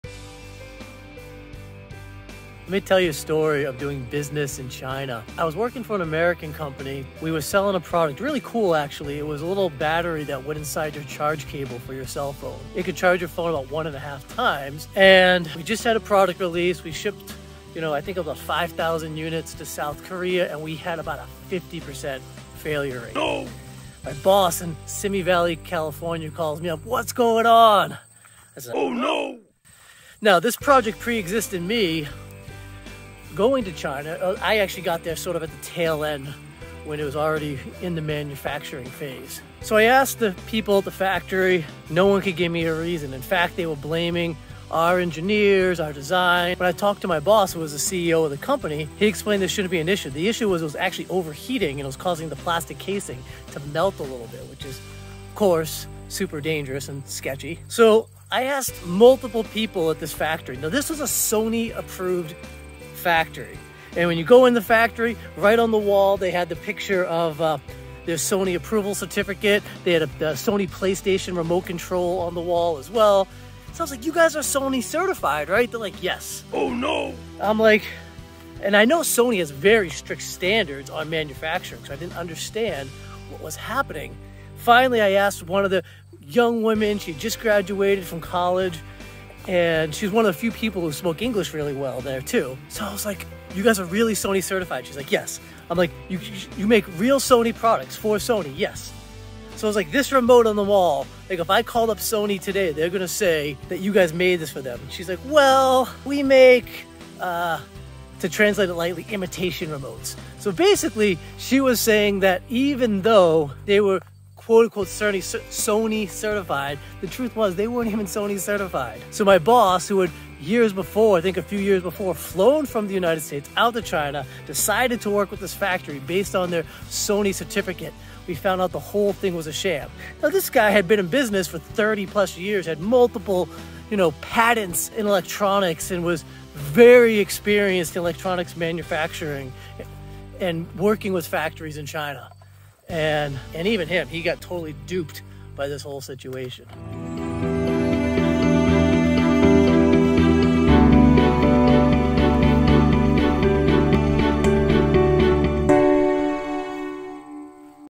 Doing Business in China
Doing Business in China - FAKE SONY!

This short video is about an experience I had in China, where my very sophisticated boss got...

Has anyone else had similar experiences?